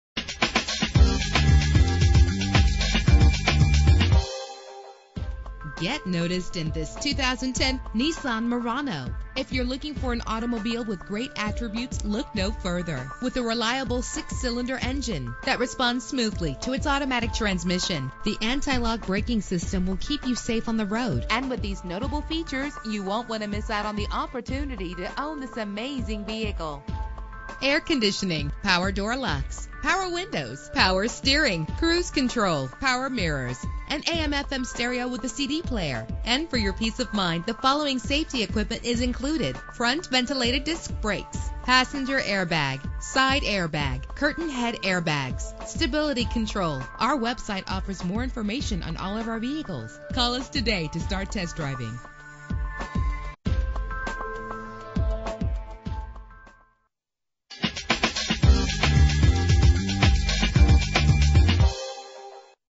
2010 Nissan Murano - ASHLAND VA
Year: 2010
Make: Nissan
Model: Murano
Trim: SL
Engine: 3.5 liter 6 cylinder 24 valve
Transmission: Automatic CVT
Color: Brilliant Silver
Mileage:
Address:
418 S WASHINGTON HWY